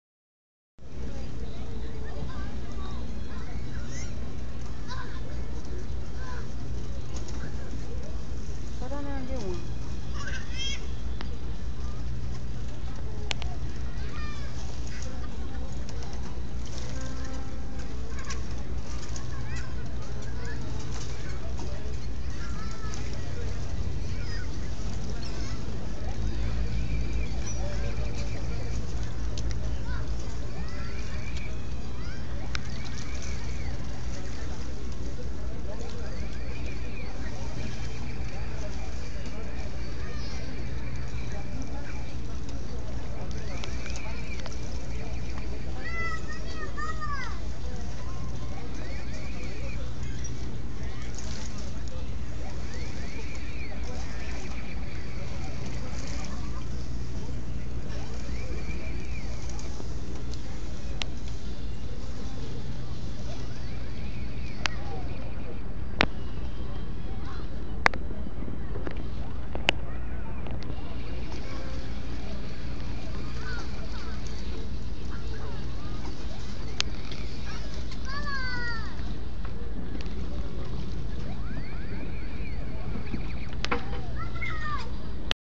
National Geographic
We at Port O'call pier out in San Pedro celebrating Ashley B-day :-) & Spotted dezz Crabs Slipping.